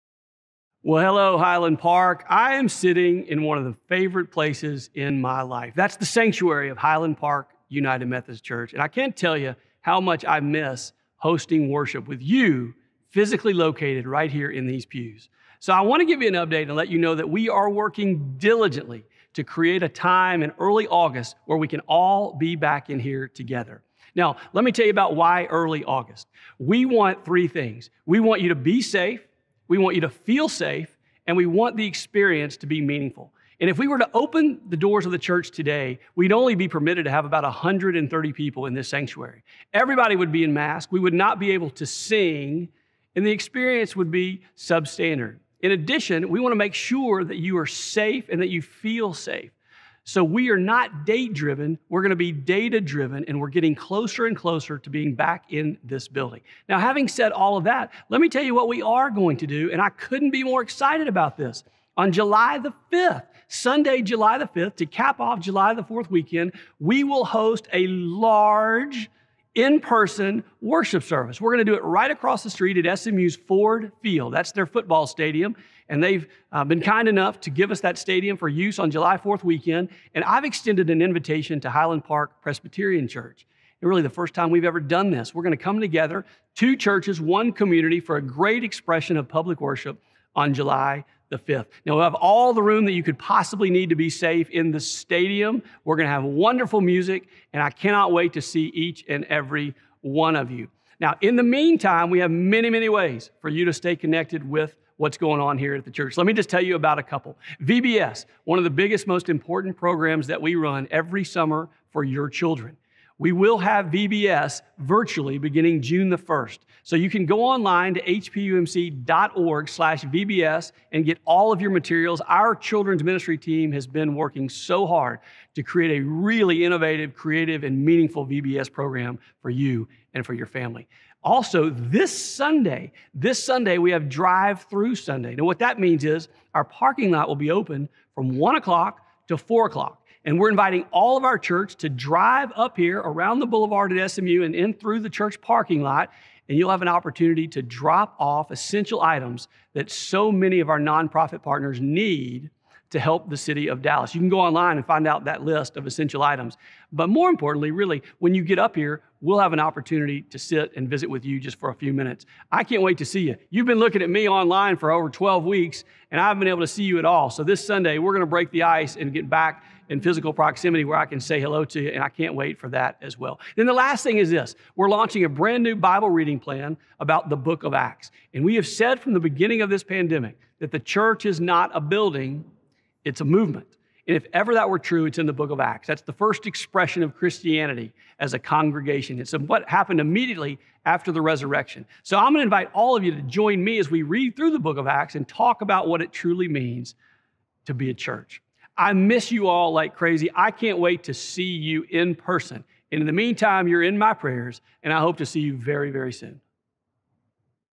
HPUMC Announces Plans for Summer of 2020
Rev. Paul Rasmussen has a special announcement about when we're hoping to be back in worship together, along with plans for a few fun things to look forward to this summer.